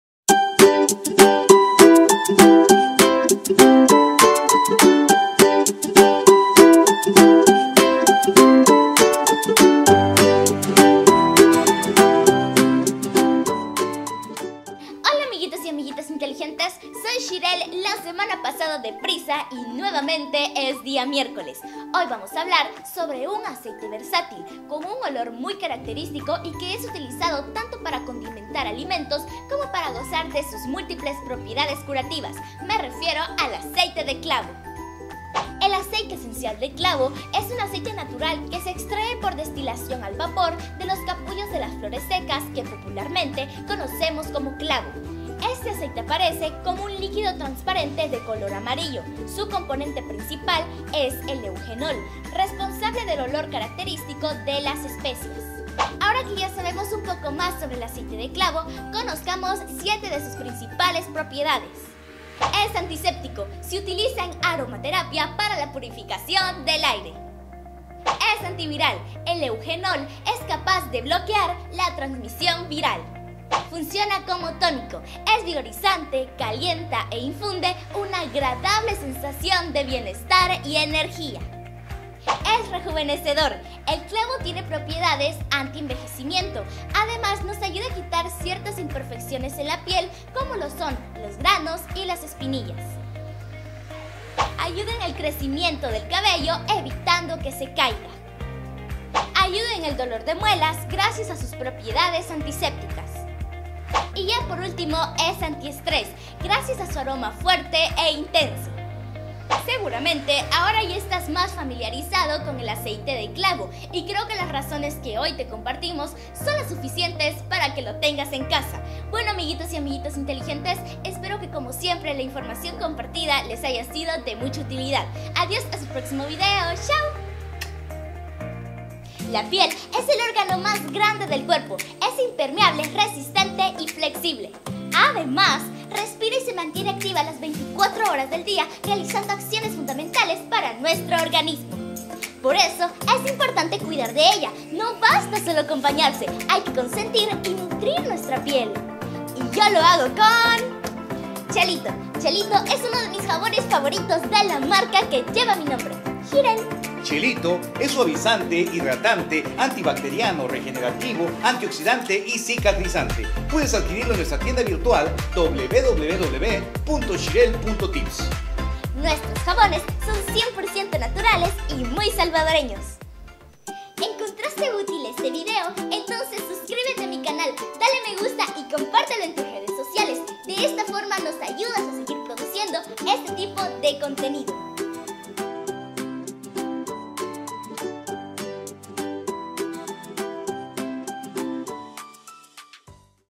Aceite de clavo ¿Existe? ¿Para qué sirve? - Shirel
El aceite esencial de clavo es un aceite natural que se extrae por destilación al vapor de los capullos de las flores secas que popularmente conocemos como clavo. Este aceite aparece como un líquido transparente, de color amarillo. Su componente principal es el eugenol, responsable del olor característico de las especias.